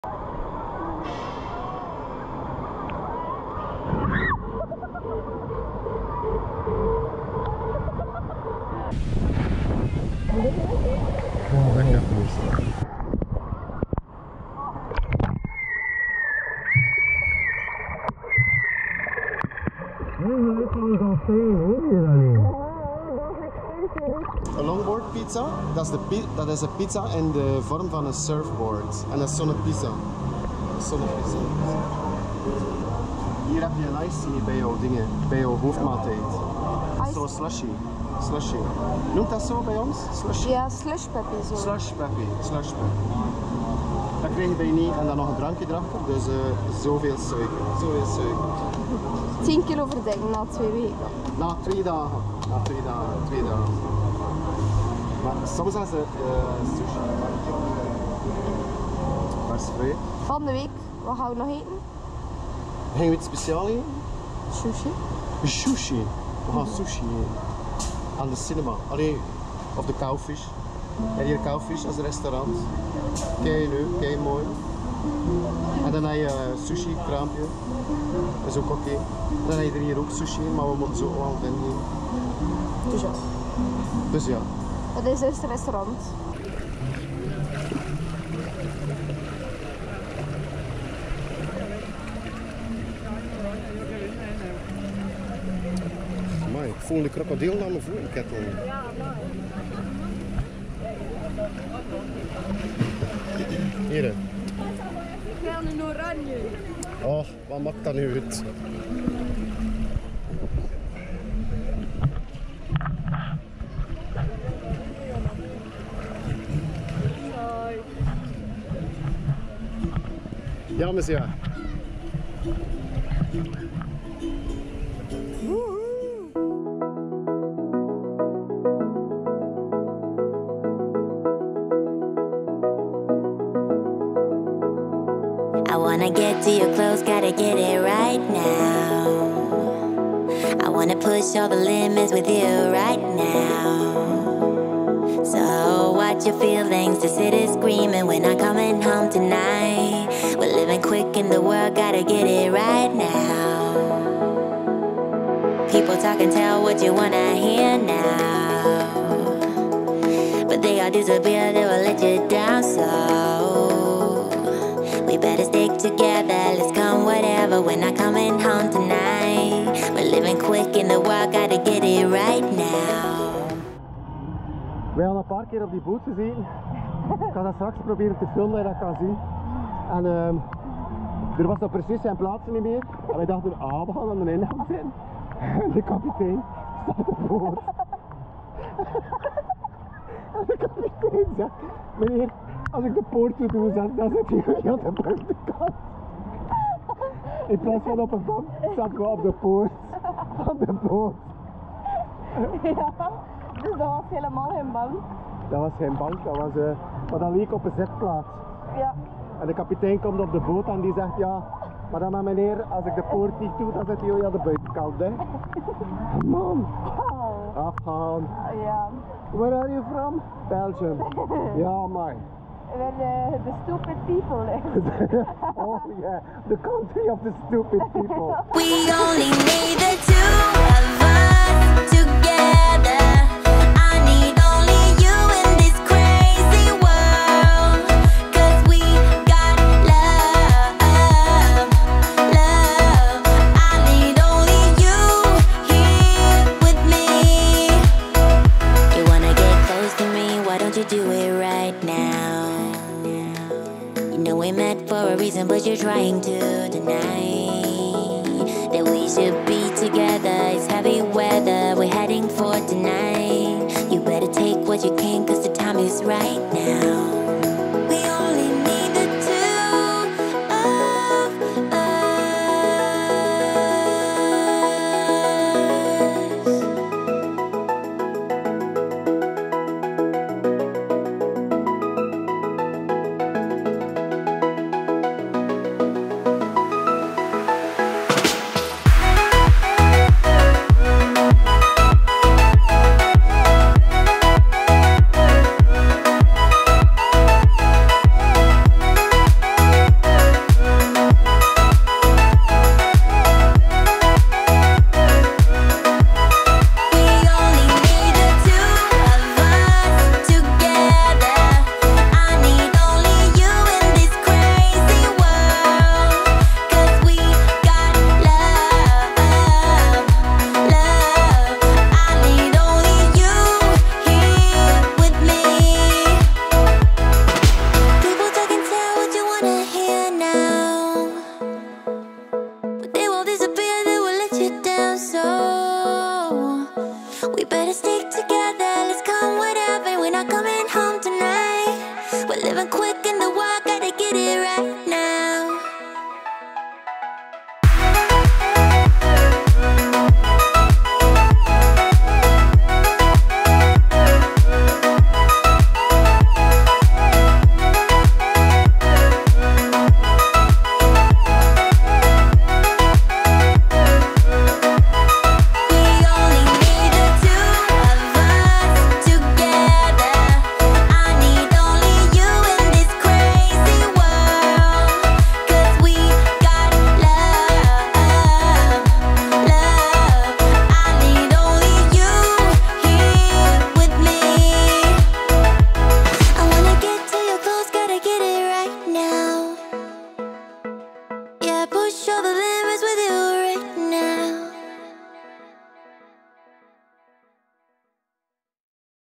Volcano Bay - Universal Orlando Resort / Plezier in de wilde rivier / Universal Studios Orlando
Last year I was with my niece at the Universal Orlando Resort and visited on a daily basis the Volcano Bay park for about two weeks. :-P This is the second video I made about Volcano Bay.

Looking for music for your YouTube channel or Facebook and still earn money?